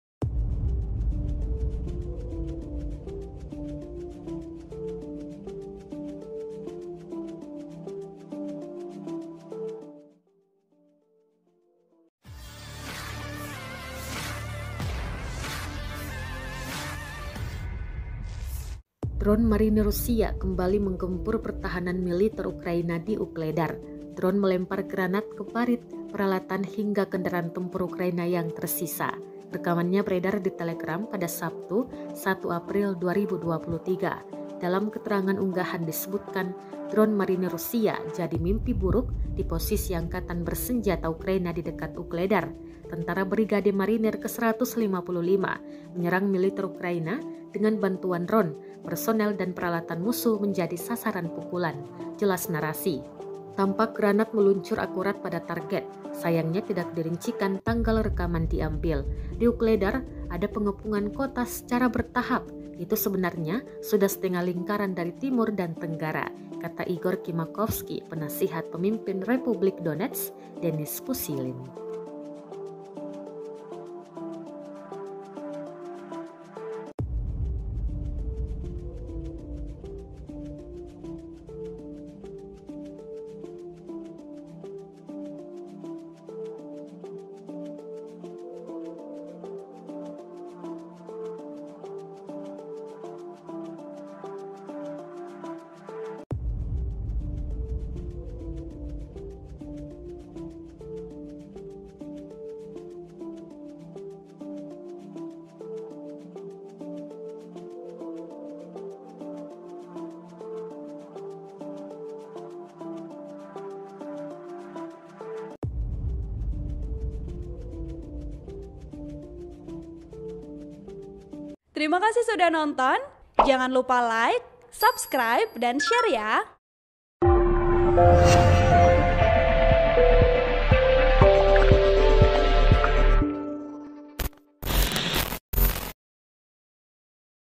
Drone Marinir Rusia Jadi Mimpi Buruk, Pertahanan Ukraina Lagi-lagi Digoyahkan di Ugledar
TRIBUN-TIMUR.COM - Drone Marinir Rusia kembali menggempur pertahanan Militer Ukraina di Ugledar.

Drone melempar granat ke parit, peralatan hingga kendaraan tempur Ukraina yang tersisa.

Rekamannya beredar di Telegram pada Sabtu, 2 April 2023.

Dalam keterangan unggahan disebutkan Drone Marinir Rusia jadi "mimpi buruk" do posisi Angkatan Bersenjata Ukraina di dekat Ugledar.

"Tentara Brigade Marinir ke-155 menyerang militer Ukraina dengan bantuan drone. Personel dan peralatan musuh menjadi sasaran pukulan," jelas narasi.

Tampak granat meluncur akurat pada target.

Sayangnya tidak dirincikan tanggal rekaman diambil.

"Di Ugledar, ada pengepungan kota secara bertahap. Itu sebenarnya sudah setengah lingkaran dari timur dan tenggara,” kata Igor Kimakovsky, penasihat pemimpin Republik Donetsk, Denis Pushilin.

Editor: Charly
Narator: Ririn